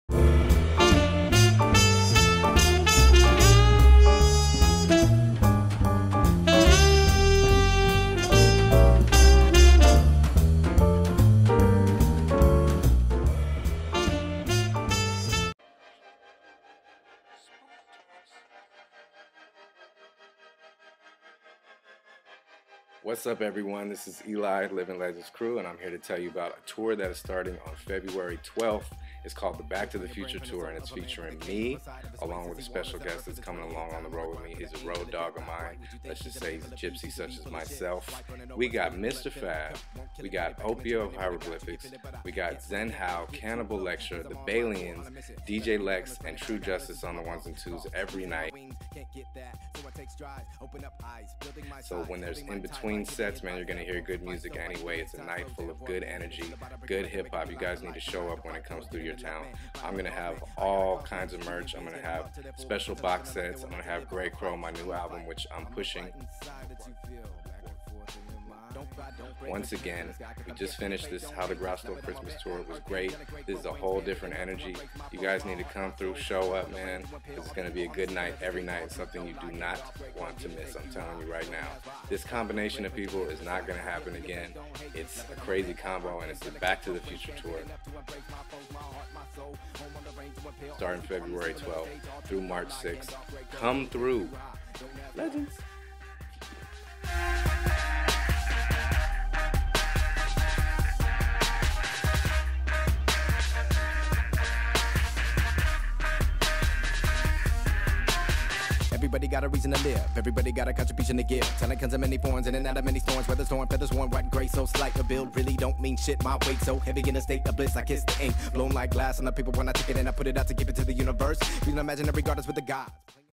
ELIGH (LIVING LEGENDS) BACK TO THE FUTURE TOUR PROMO
Eligh speaks about THE BACK TO THE FUTURE TOUR and his brand new album. buy that bad boy on itunes now or when you see him...support independent musicians...we do this for yall